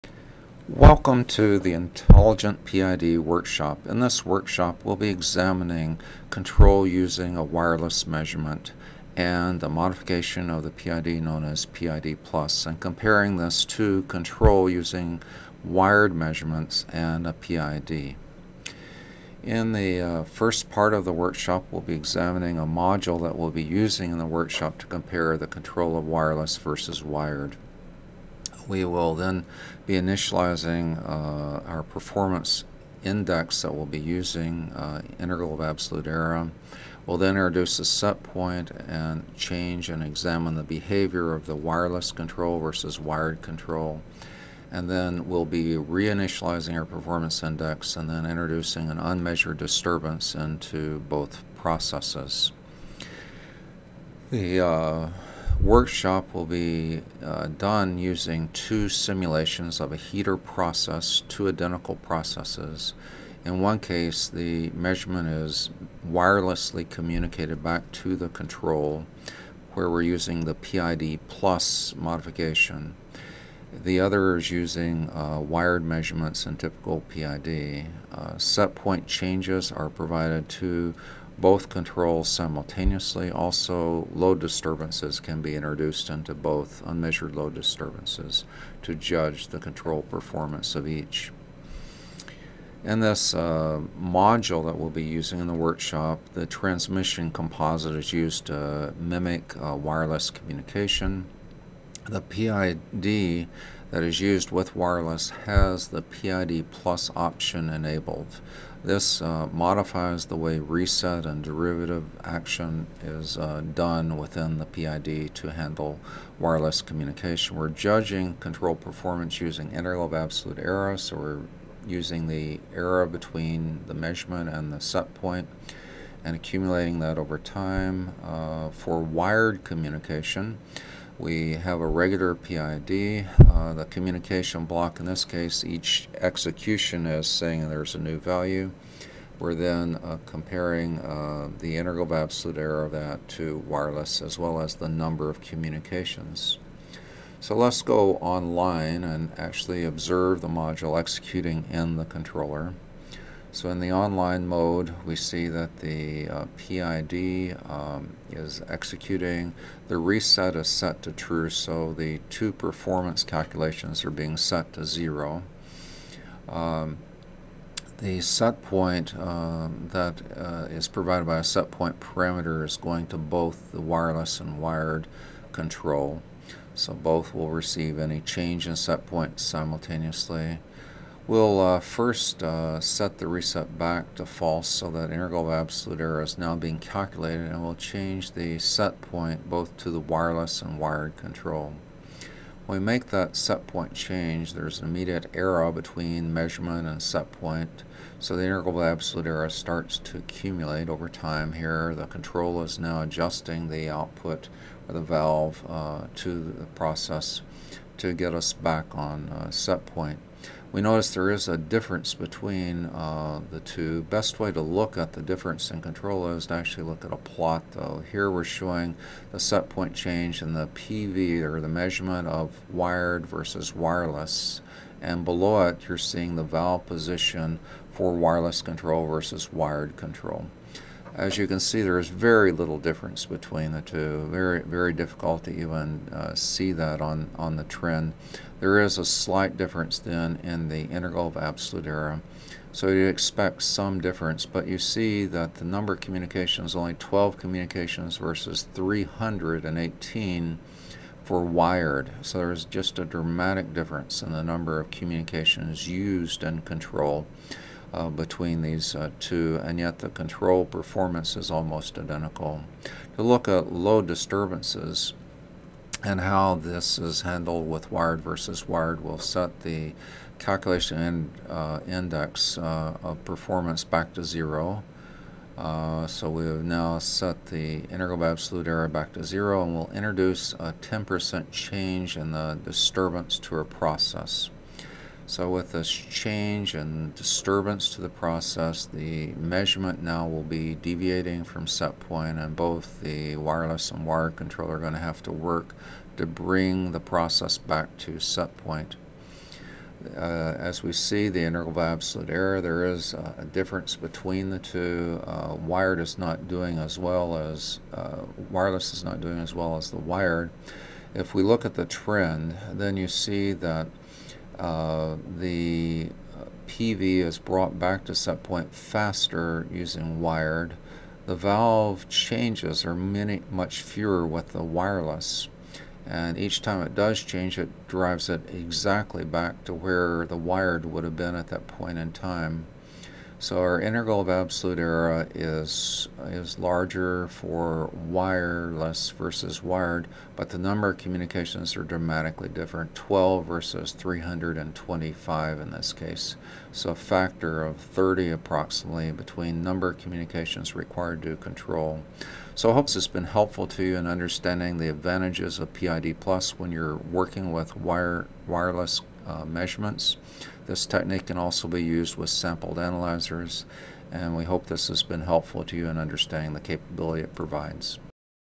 Intelligent PID Workshop
This video shows the solution to the intelligent PID workshop contained in the book Advanced Control Foundation. Anyone can access the book's web site to view the workshop exercise and process description.